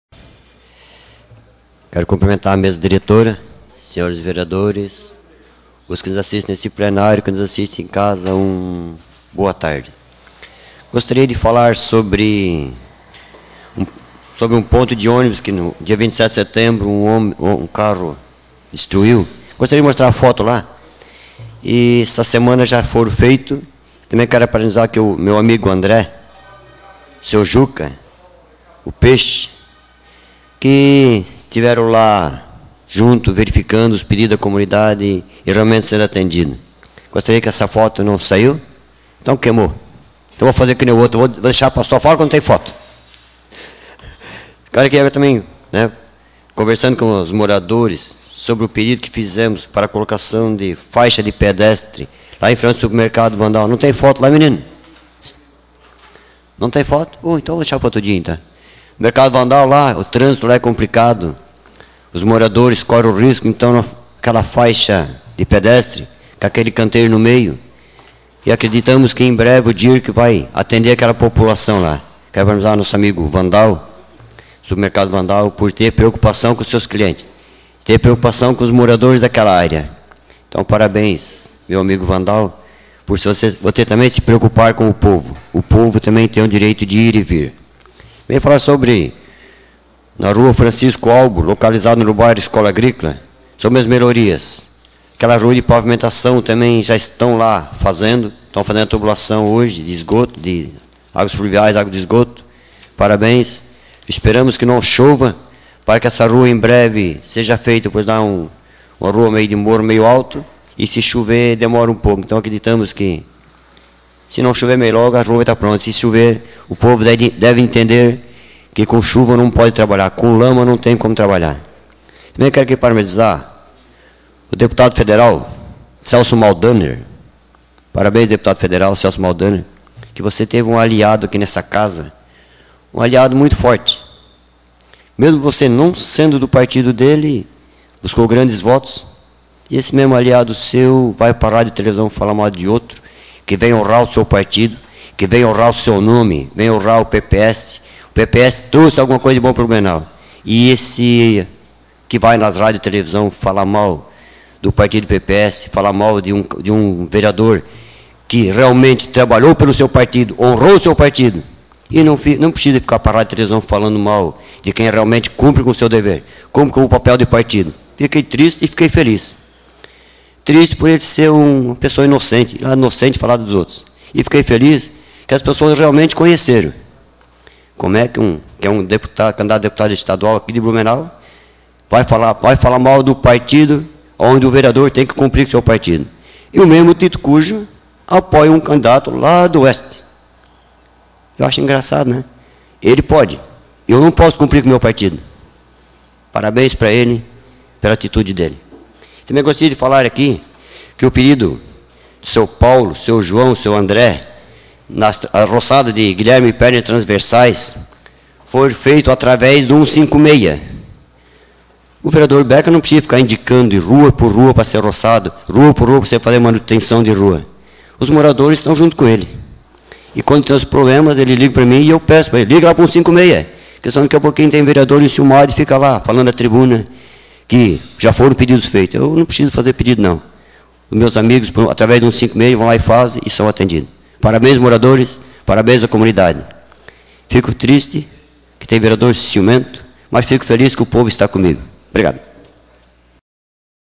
Pronunciamento Vereador Oldemar Becker - 09/10/14
O parlamentar Oldemar Becker (PPS) falou do pedido feito pelos moradores para a colocação de uma faixa de pedestres em frente ao supermercado Wandall, na rua Guilherme Poerner, bairro Passo Manso. O vereador comentou ainda sobre as obras de melhorias na rua Francisco Albo, no bairro Escola Agrícola. Disse que espera que o tempo colabore para que a entrega à comunidade seja feita em breve.

 Fonte: Assessoria de Imprensa Camarablu

Foto: Renan Olaz | Agência Camarablu